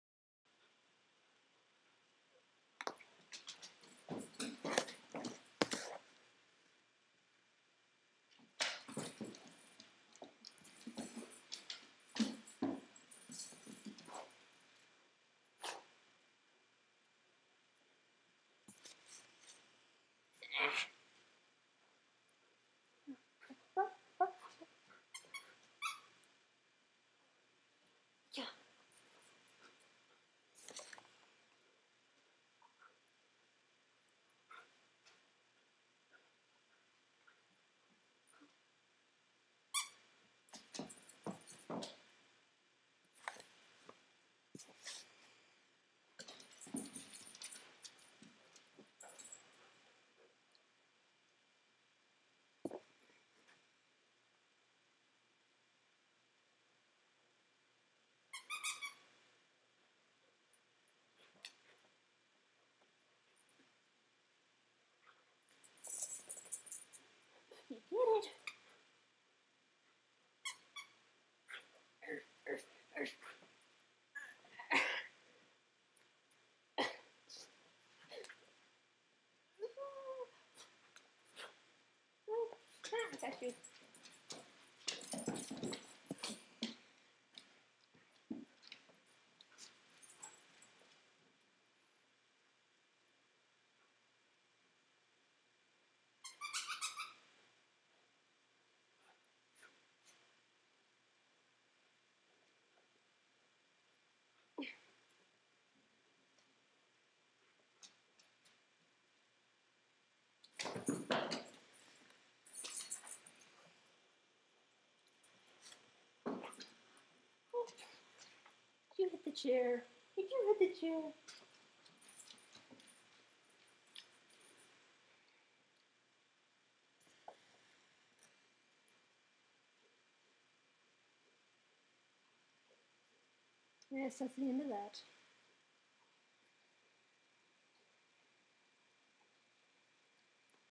JUNO'S A PUPPY!!!!!!! ...for about two minutes.
Juno likes to act like a puppy for short bursts of time. Usually she gets bored and goes back to being a tired almost-12-year-old dog. It's adorable but also kind of confusing. Oh, well.